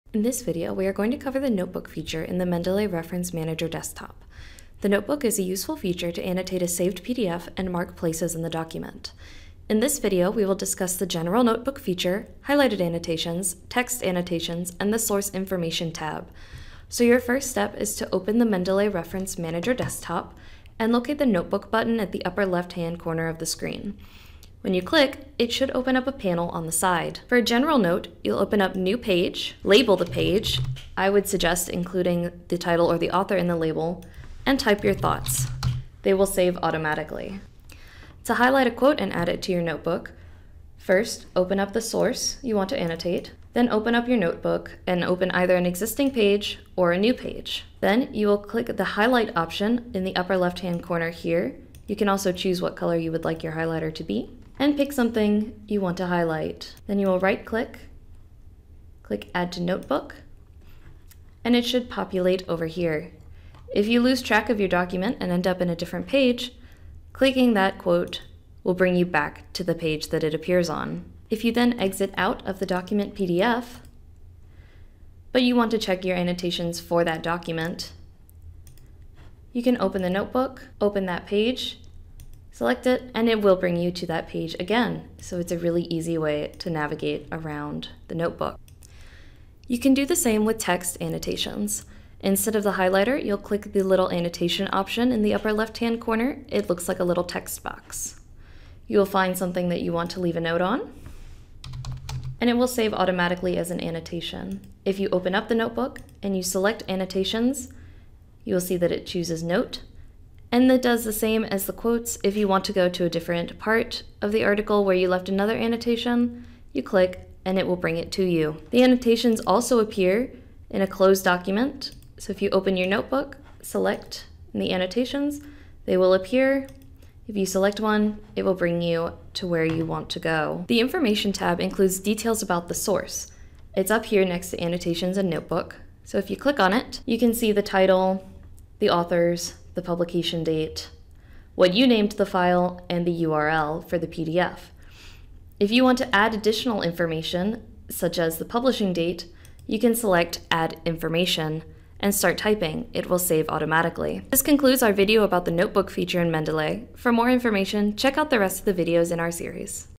Mendeley Notebook Feature
This video covers the Notebook feature of the Mendeley Reference Manager, which allows the user to make annotations, highlight and save quotes, and reference information about their source.

Scripted and narrated by Margaret Bates